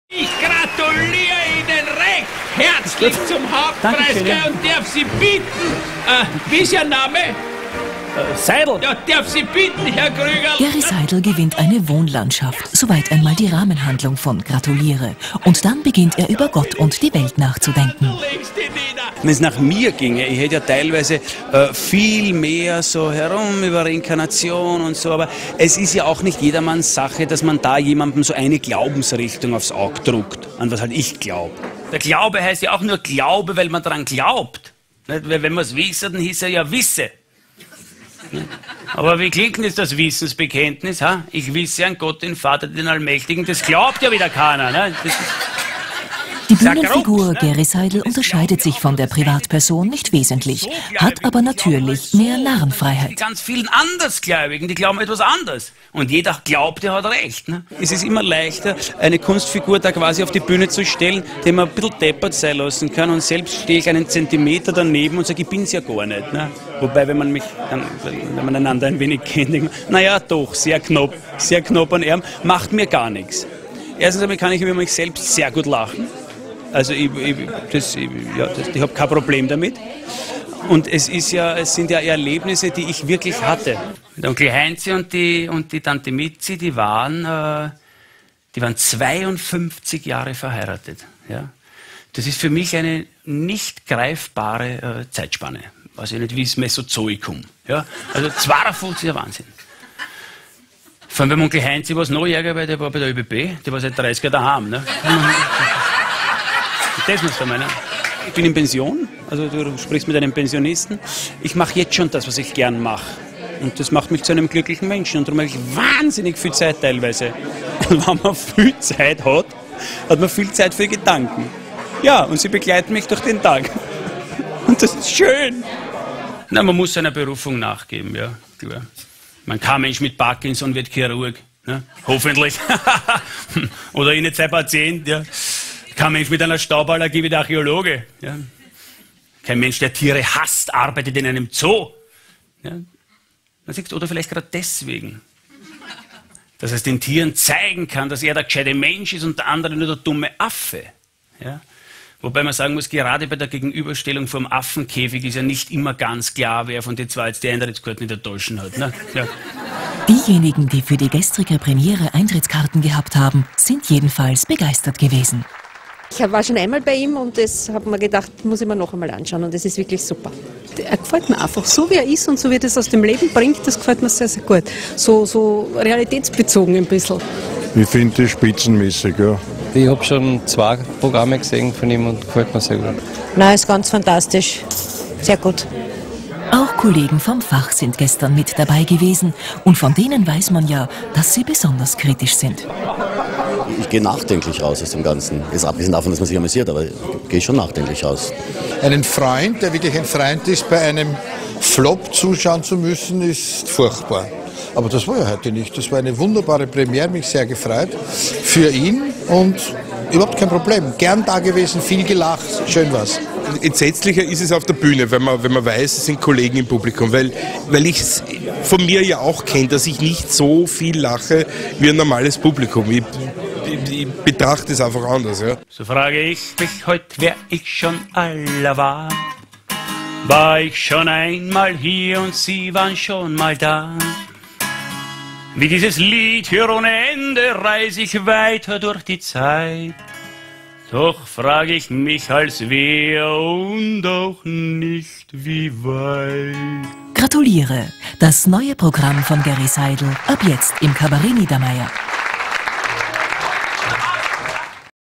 Gery Seidl Gratuliere Premiere
(c) ORF, TV-Beitrag Kabarett Gery Seidl, Premiere "Gratuliere!", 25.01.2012, Kabarett Niedermair.